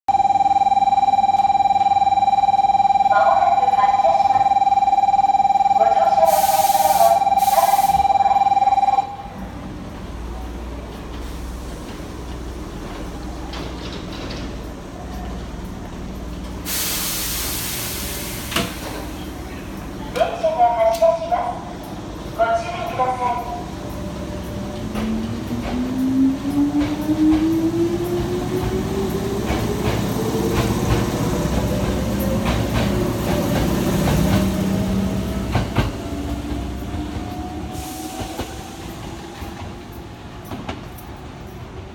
阪急6000系6022F 今津線 西宮北口駅発車
この編成はワンマン運転に対応しているので、運転士がドアの開閉も行っています。